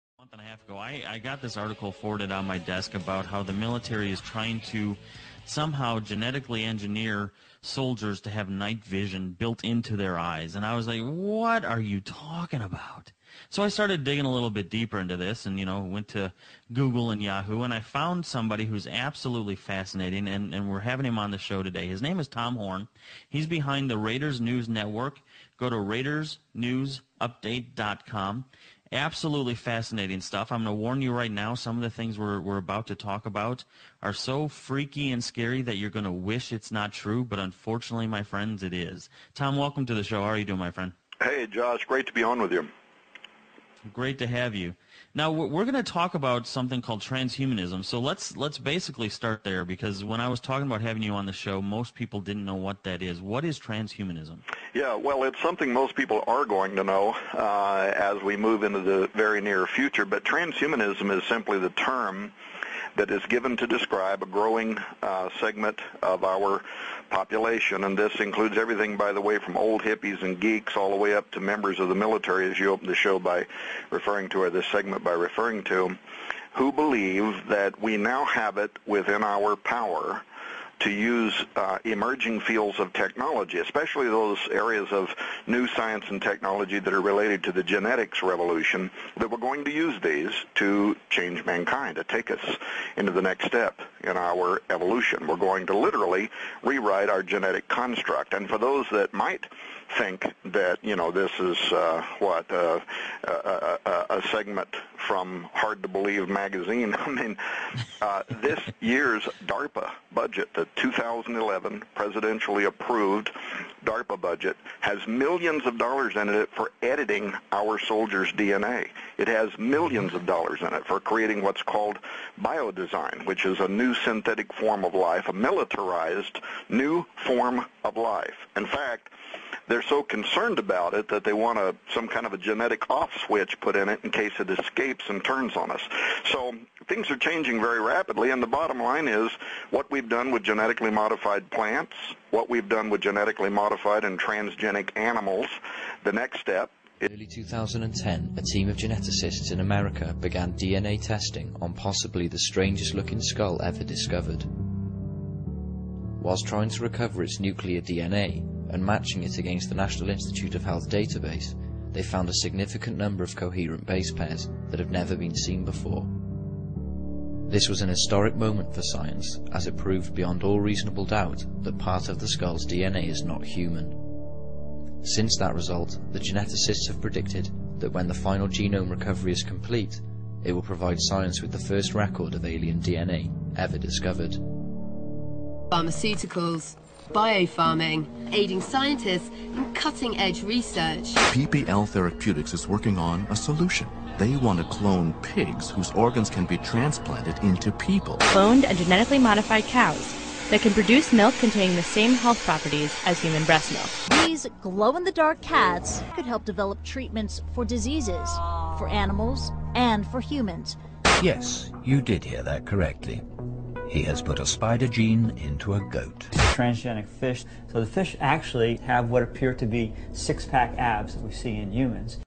Get your new DNA BUDDY
DNA projects for your kids It's so NEAT. DNA research saves lives. DNA for kids. Ever seen a dogs head brought back to life?? He can occupy your kids, cook dinner, clean the house, do the laundry, do the shopping...so you don't have to be tracked all the way to the store and back ...the uses go on and on and on; especially for DARPA military projects. IN FACT ...Who the fuck needs a sheep population when you can have a DNA BUDDY